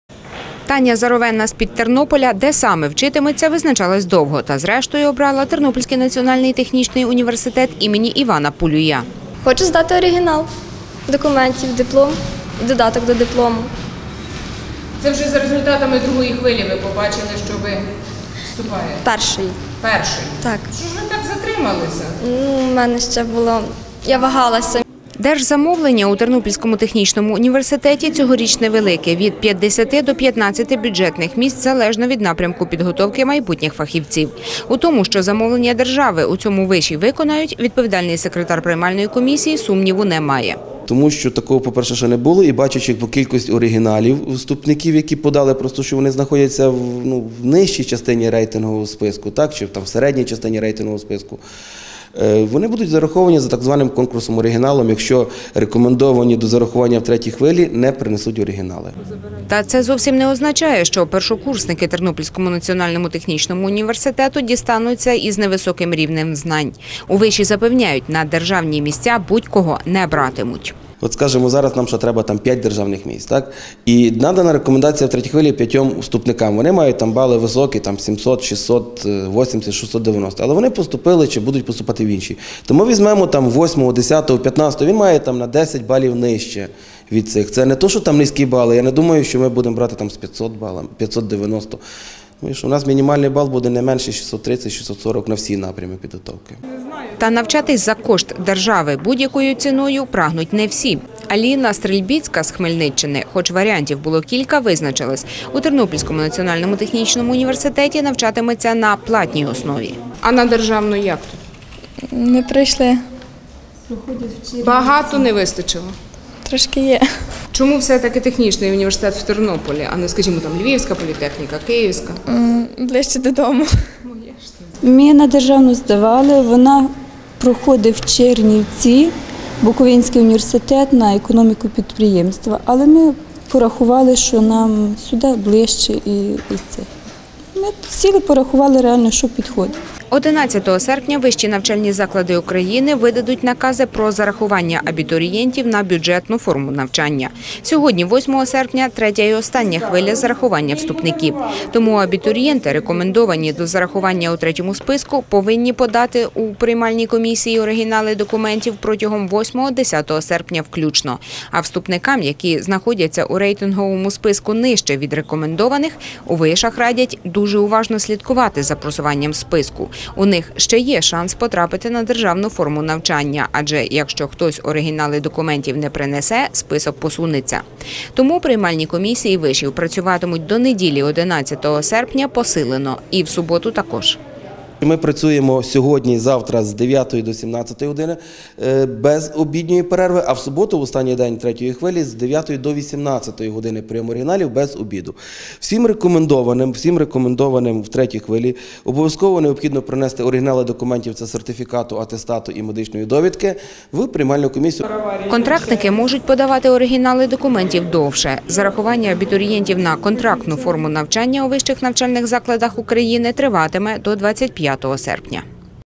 8 серпня ВНЗ оприлюднили третій список вступників
Вступна кампанія-2013 вийшла на фінішну пряму. Сьогодні до 12-ї години вищі навчальні заклади України оприлюднили третій список вступників, рекомендованих до зарахування за гроші державного бюджету. Під час «третьої хвилі» з рейтингових списків виключають вступників, які були рекомендовані до зарахування, але з 5 до 7 серпня не подали оригінали документів до вищого навчального закладу. У підсумку їх місця зайняли абітурієнти, які знаходились у списку нижче. Сьогодні - остання «хвиля» зарахування вступників. Абітурієнти, рекомендовані до зарахування у третьому списку, повинні подати до приймальних комісій необхідні оригінали документів до 10 серпня включно. А вже у неділю, 11 серпня, вищі навчальні заклади України мають видати накази про зарахування абітурієнтів на бюджетну форму навчання.